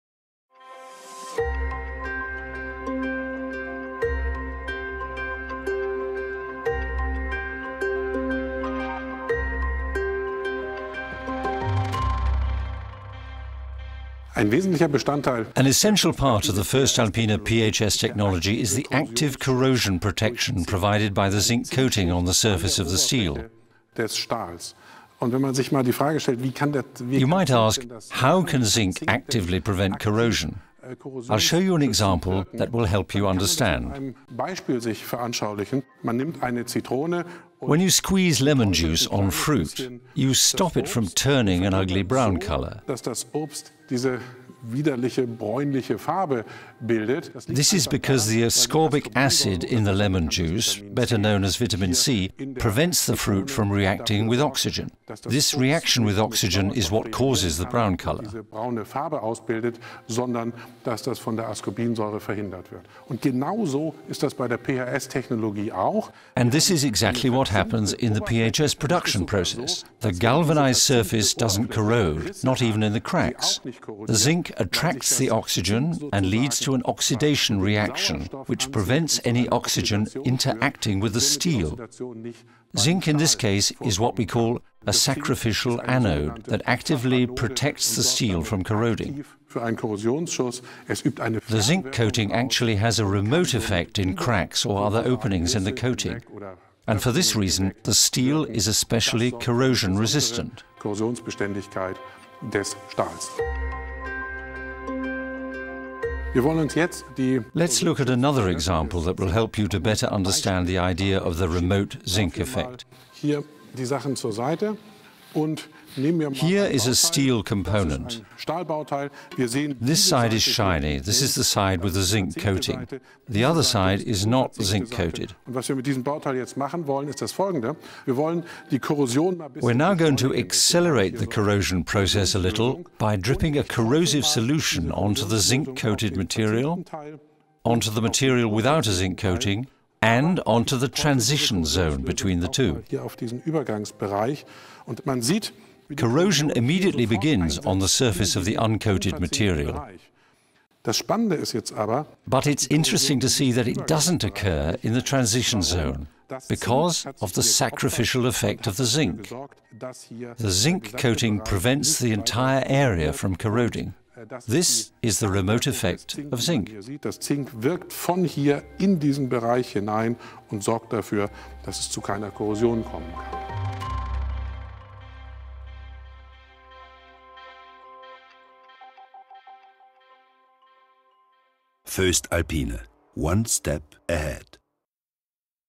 phs secret: The remote effect of corrosion-resisting zinc, with Professor M. Tolan
What does corrosion resistance of the voestalpine phs technology have to do with a lemon? Metin Tolan, professor of experimental physics, shares the secrets of phs technology in surprising culinary analogies. phs technology features the hot forming of hot-dip galvanized steel strip to be used in corrosion-resistant press-hardened components.